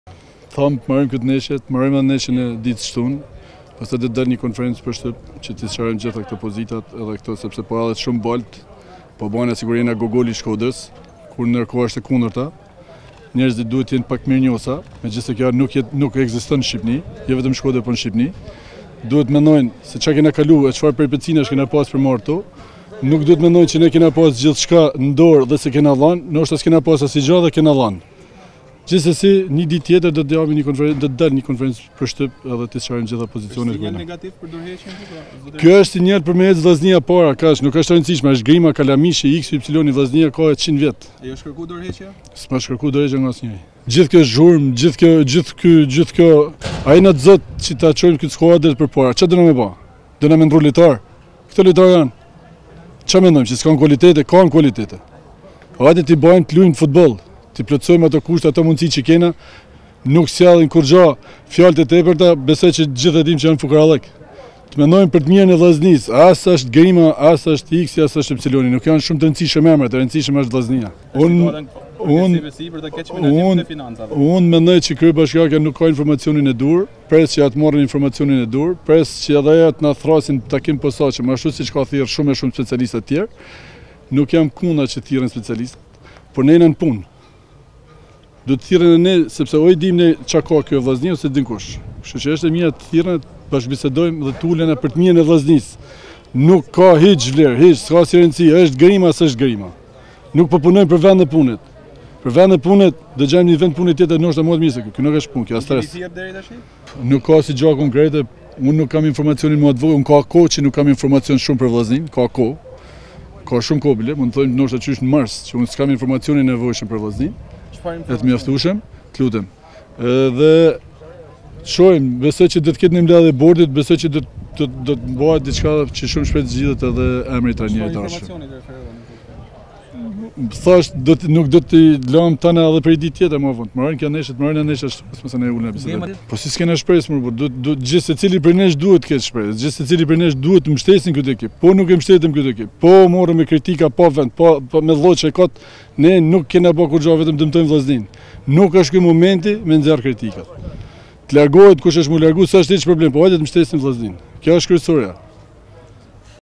GRIMA: NUK DORËHIQEM, KRYEBASHKIAKJA S’KA INFORMACION
GRIMA: S’MA KANË KËRKUAR DORËHEQJEN, QË NË MARS S’DI ASGJË